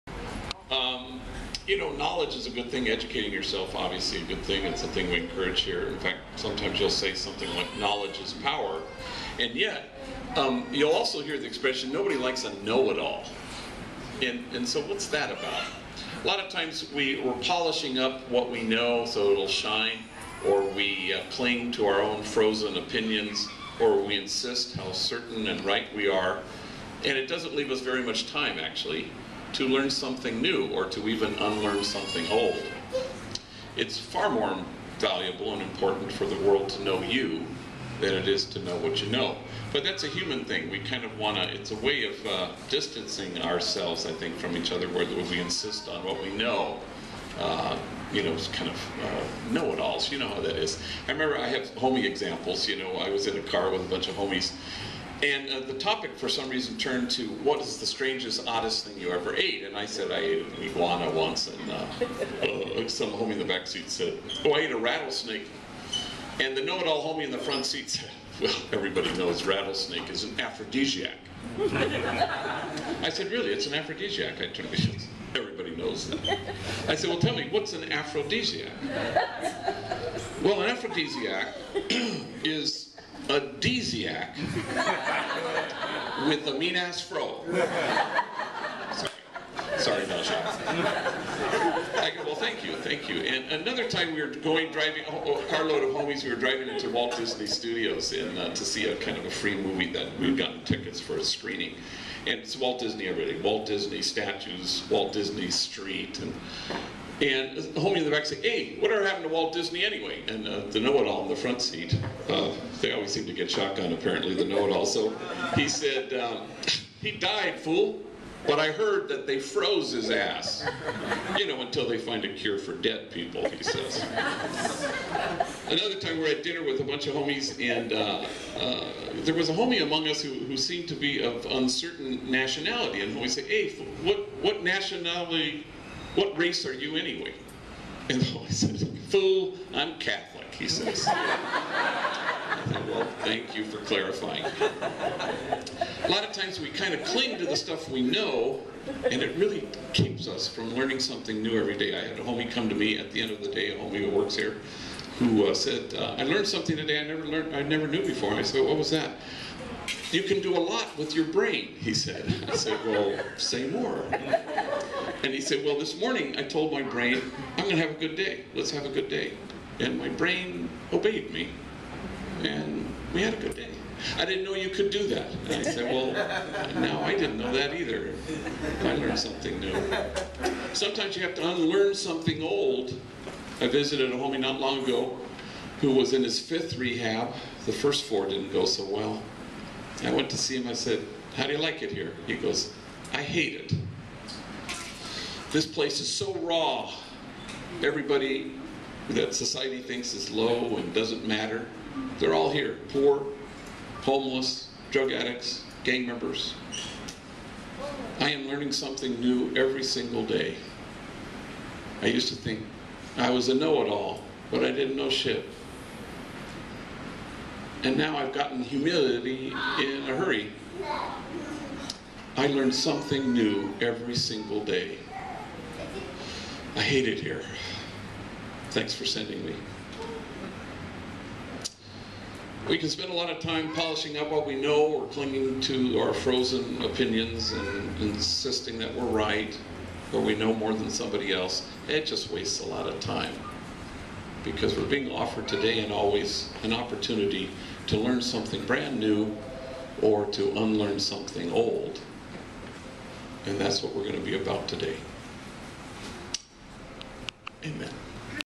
Father Greg Boyle Thought of the Day
"G" speaks about the difference between learning something new every day, and being a know-it-all.